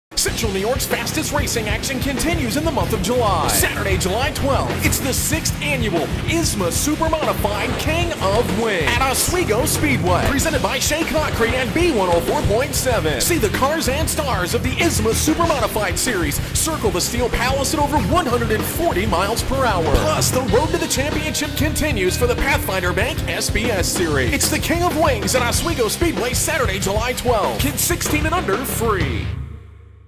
Oswego Speedway - King of Wings VI - July 12, 2014
Join us Saturday, July 12 for King of Wings VI featuring the ISMA Supermodifieds!  One of the biggest winged Supermodified events of the year is a can't miss event at Oswego Speedway!  or www,ismasupers.com.